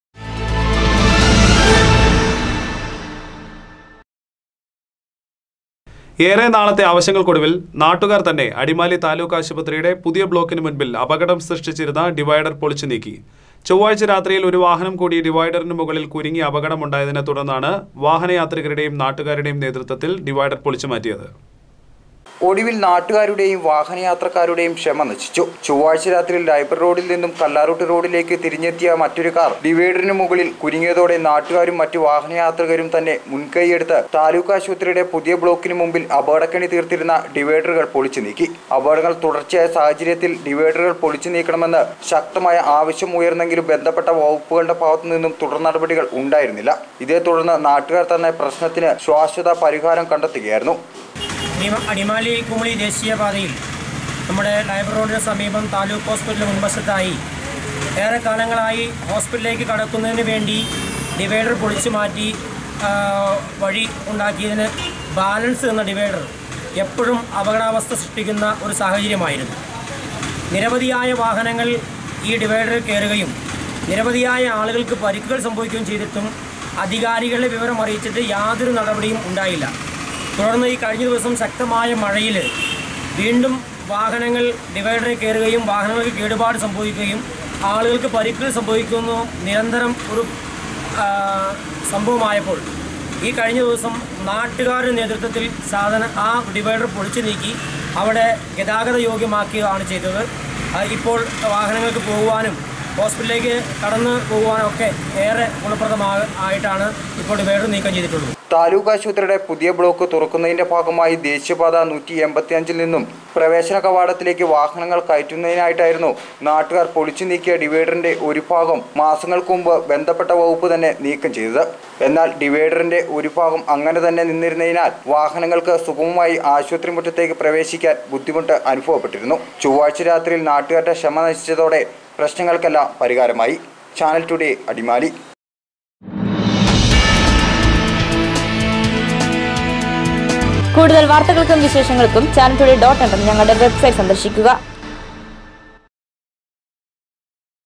താലൂക്കാശുപത്രിക്ക് മുമ്പില്‍ അപകടം സൃഷ്ടിച്ചിരുന്ന ഡിവൈഡര്‍ നാട്ടുകാര്‍ പൊളിച്ച് നീക്കി.
ഏറെ നാളത്തെ ആവശ്യങ്ങള്‍ക്കൊടുവില്‍ നാട്ടുകാര്‍ തന്നെ അടിമാലി താലൂക്കാശുപത്രിയുടെ പുതിയ ബ്ലോക്കിന് മുമ്പില്‍ അപകടം സൃഷ്ടിച്ചിരുന്ന ഡിവൈഡര്‍ പൊളിച്ച് നീക്കി.ചൊവ്വാഴ്ച്ച രാത്രിയില്‍ ഒരു വാഹനം കൂടി ഡിവൈഡറിന് മുകളില്‍ കുരുങ്ങി അപകടമുണ്ടായതിനെ തുടര്‍ന്നാണ് വാഹനയാത്രികരുടെയും നാട്ടുകാരുടെയും നേതൃത്വത്തില്‍ ഡിവൈഡര്‍ പൊളിച്ച് മാറ്റിയത്.